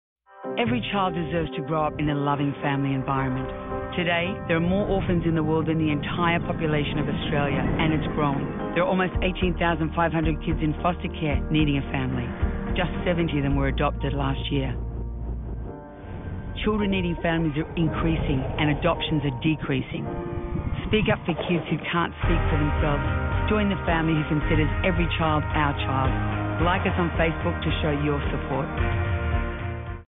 National Adoption Awareness Week CSA
National Adoption Awareness Week Community Service Announcement - Narrated by Deborra-lee Furness.